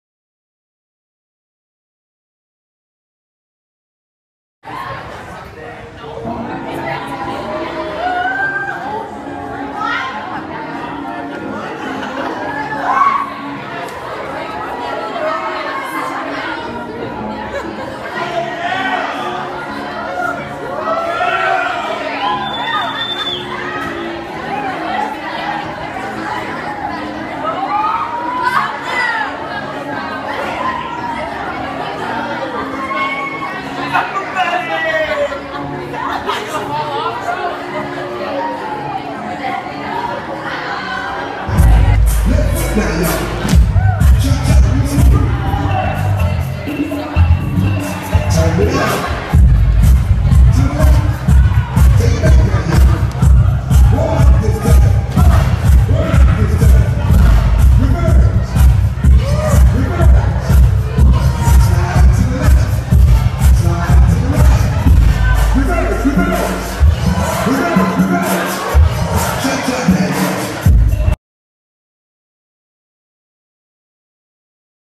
Zeeland East 2015 Prom
Students ride carousel, dance at "Wonderland Prom" 2015